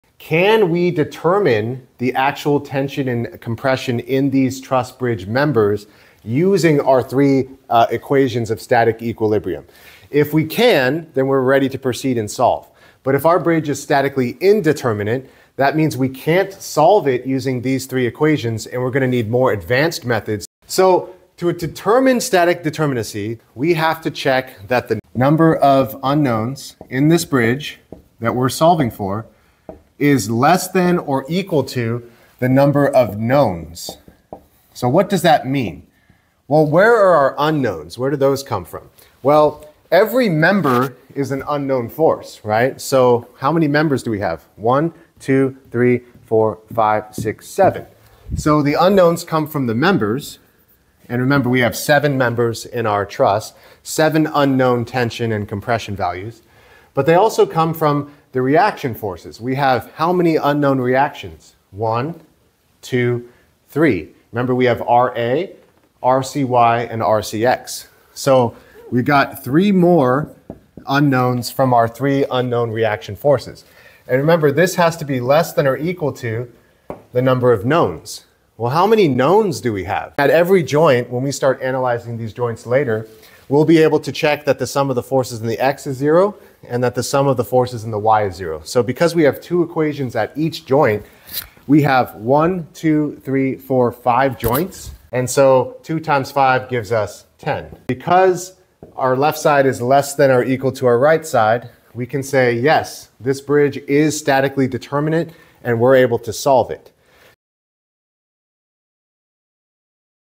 Static Equilibrium: Understanding Forces on Bridges
Can this truss bridge actually be solved with static equilibrium? The secret lies in unknowns vs. knowns. See if the bridge is 'statically determinate' or needs advanced methods to solve! Is bridge-solving possible? It depends!